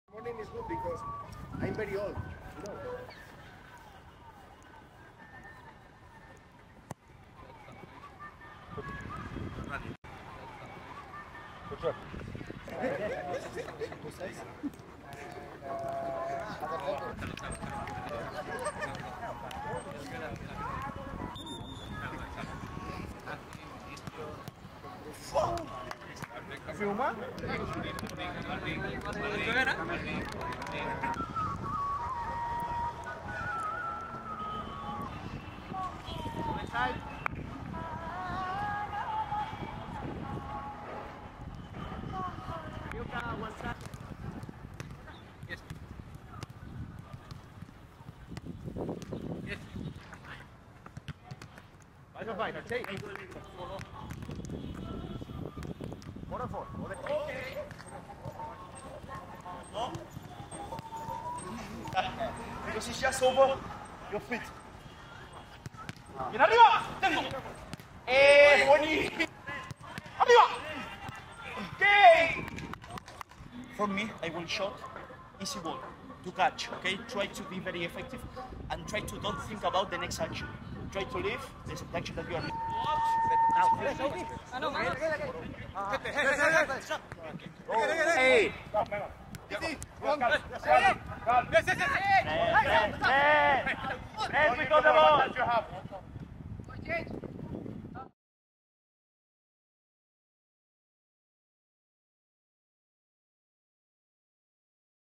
AFC Cup | Odisha FC Training Session MD-2 | Bhubaneswar | 2023-24
Watch the KalingaWarriors get back to work in Bhubaneswar ahead of our return to AFCCup action 🟣⚫️